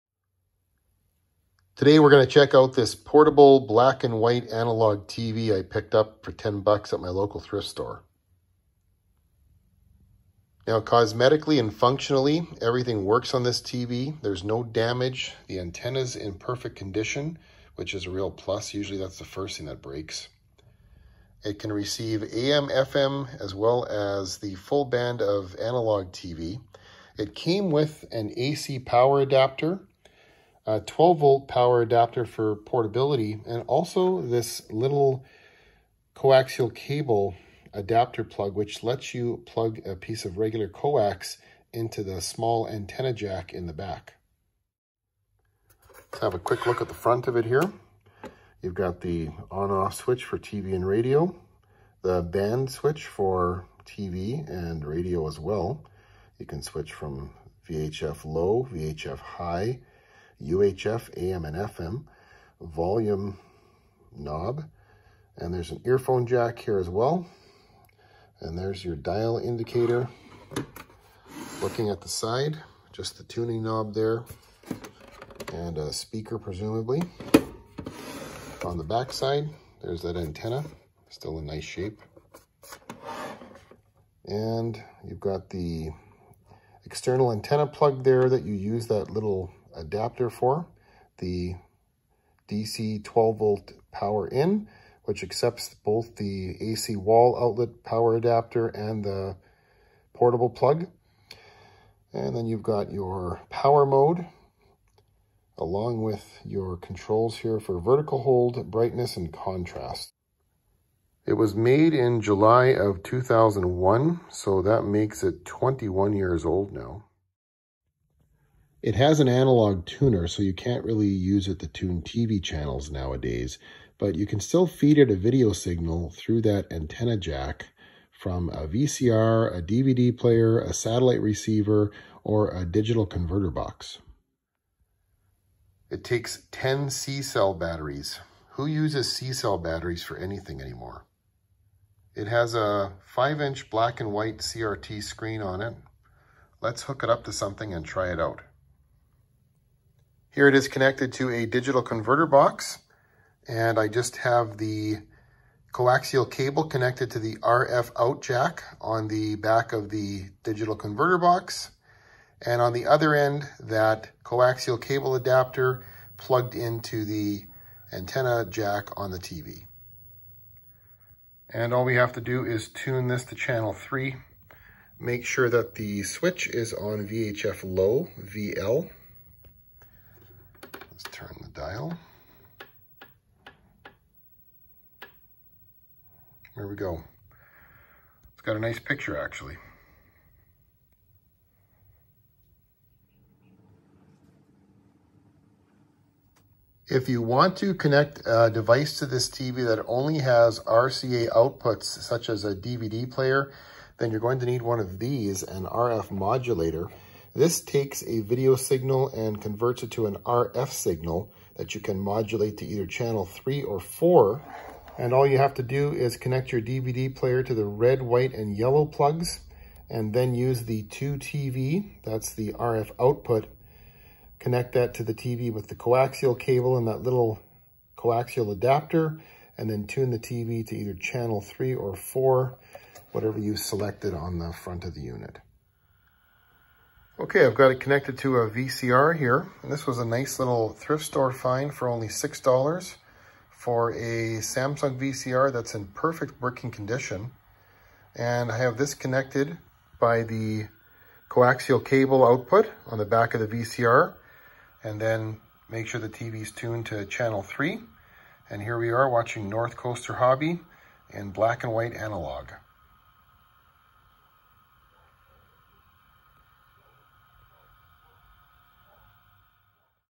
Portable Black and White Analog TV - a cool thrift store find
Portable Black and White Analog TV - Analog Television B&W TV
A portable black-and-white analog television I picked up from the local thrift store. It came with an AC adapter, a DC plug for the car, as well as a coaxial cable to ⅛” antenna plug.  You can connect any RF-modulated device to it, or use an external RF modulator to connect a DVD player or other RCA-output devices to this relic from a (not-so) distant era of TV. Check out some other videos I have made about vintage portable analog TV’s below:

Connecting a Satellite receiver to a second TV with an RF modulator and IR remote extender